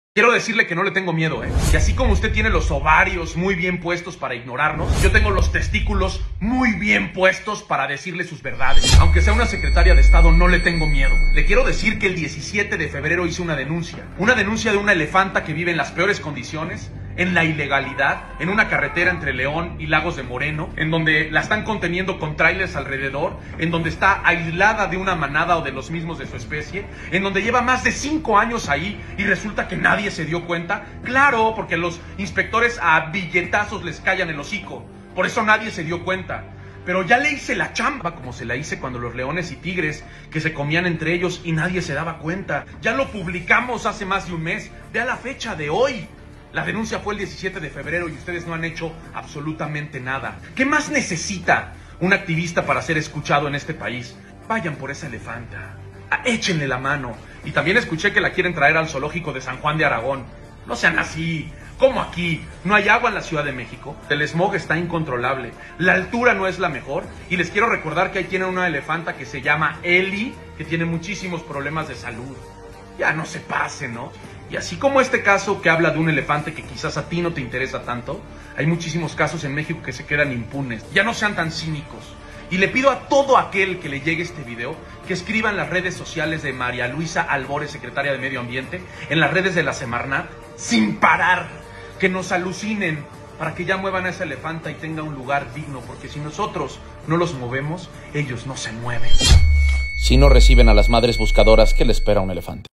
LA VERDAD PESA MÁS QUE CUALQUIER EXCUSA!!
'La verdad pesa más que cualquier excusa, más de un mes y no chambean' @mary_luisa_ag afortunadamente ya va a terminar su ad...mn pero no se nos va a olvidar su indiferencia, el problema, no es un problema de partido, ni de colores, ni banderas, le.mp4